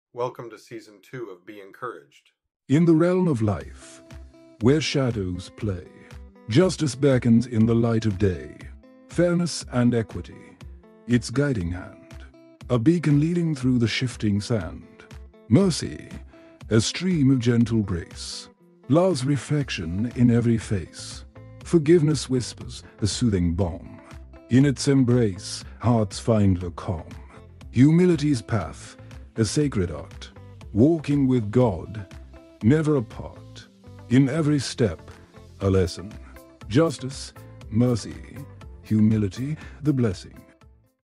Justice
Justice, Mercy, Humility

In the realm of life, where shadows play,

Justice beckons in the light of day.

Fairness and equity, its guiding hand,

A beacon leading through the shifting sand.

Mercy, a stream of gentle grace,

Love's reflection in every face.

Forgiveness whispers, a soothing balm,

In its embrace, hearts find a calm.

Humility's path, a sacred art,

Walking with God, never apart.

In every step, a lesson,

Justice, mercy, humility, the blessing.